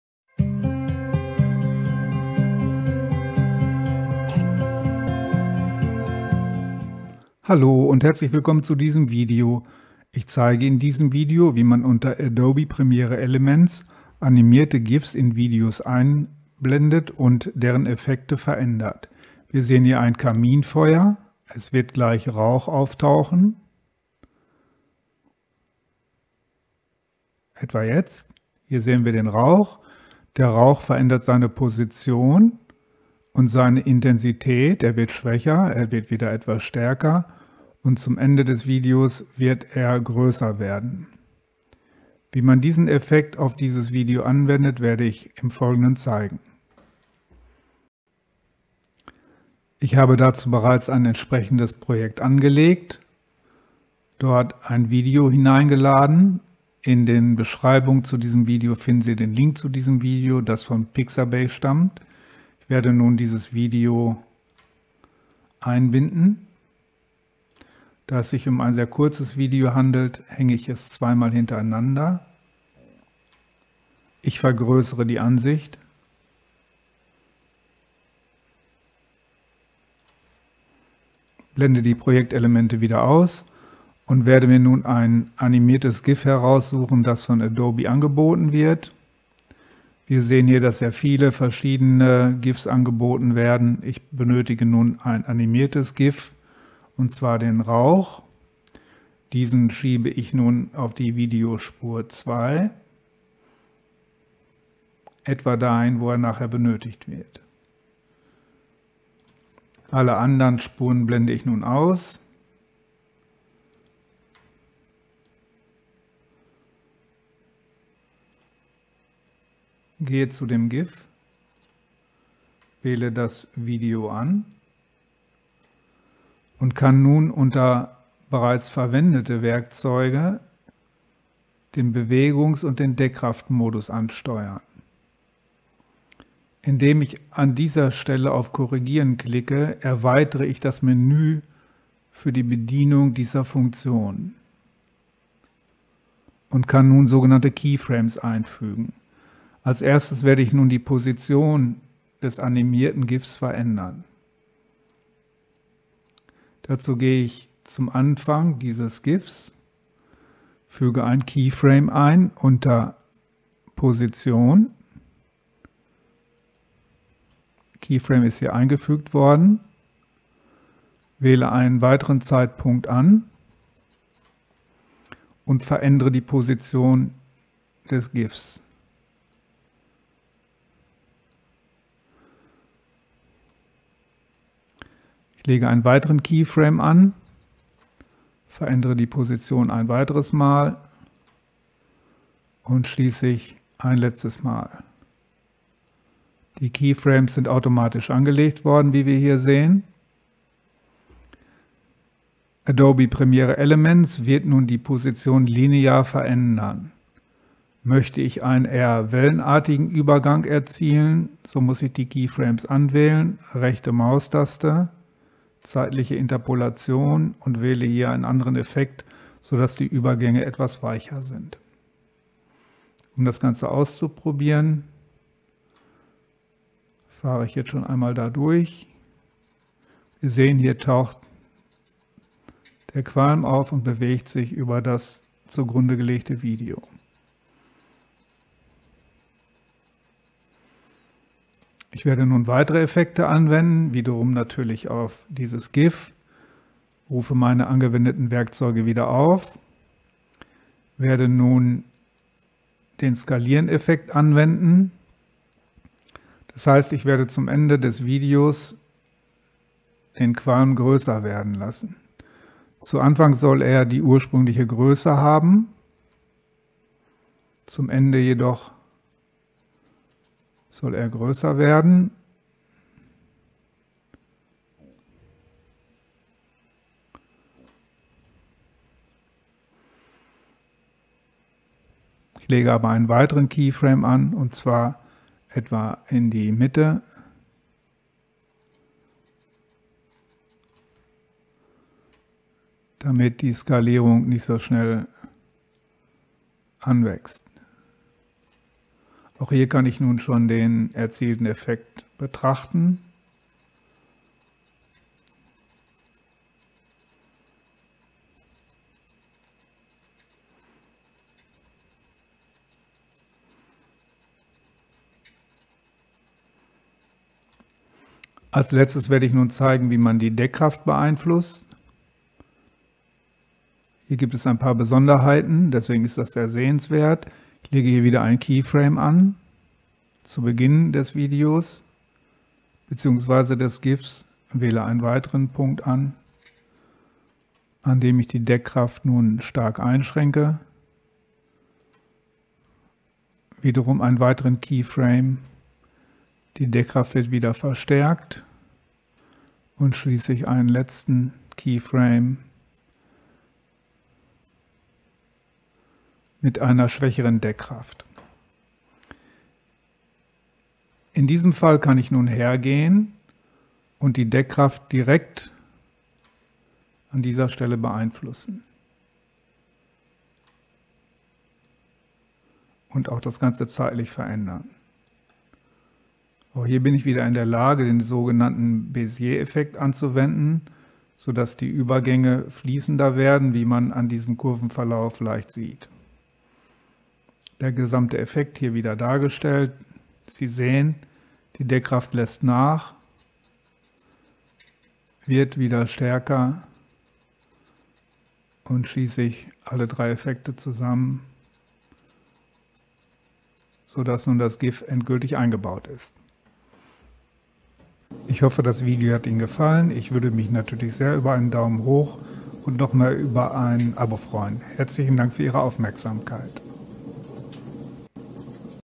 Animation mit Adobe Premiere Elements / Animierte GIF anwenden und mit Keyframes bearbeiten
Blenden Sie einfache Animationen in Ihre Videos ein und bearbeiten Sie diese professionell mit Hilfe von Keyframes.

Video "Fireplace 1971"  mit Kaminfeuer von Vladimir Maric auf Pixabay